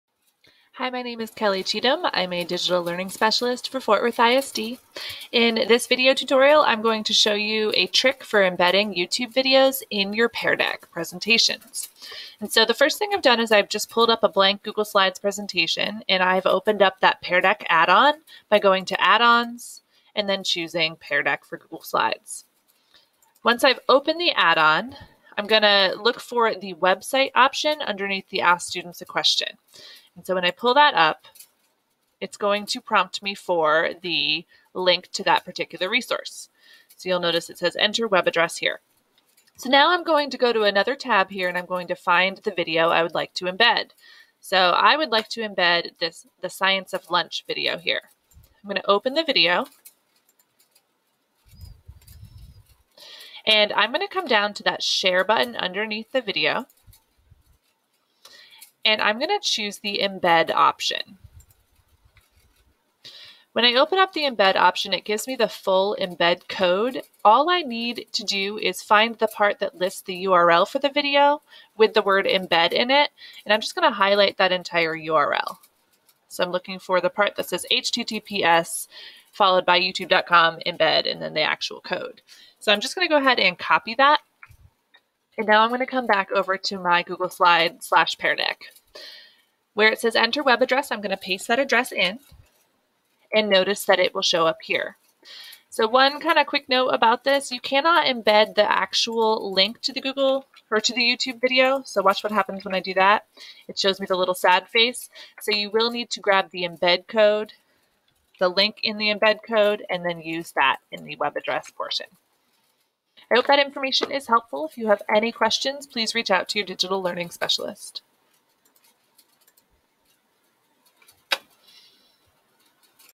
Embedding YouTube Videos in Pear Deck
When you try to embed a YouTube video into a Pear Deck presentation, you cannot use the actual URL. Instead, use this trick to find and use the embed URL for the video.